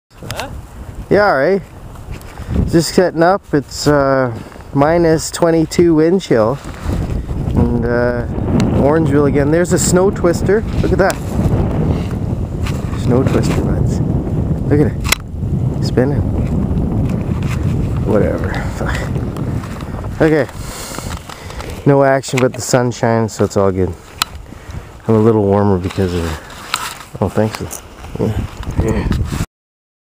Orangeville cooooooooooooold -22
ice fishing at island lake ... colderd struck out